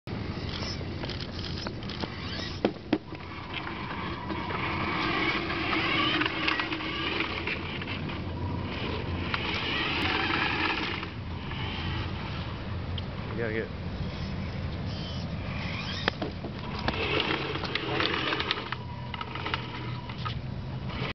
UC Berkeley ME102 - BearClaw - Tennis Ball Collector (2/4)
Bearclaw picks up two balls and heads for a third.

Mechanical Engineering 102 - Mechanical Engineering Design
University of California, Berkeley
Fall 2009 - Senior Design Project

Automatic Tennis Ball Collector "BearClaw"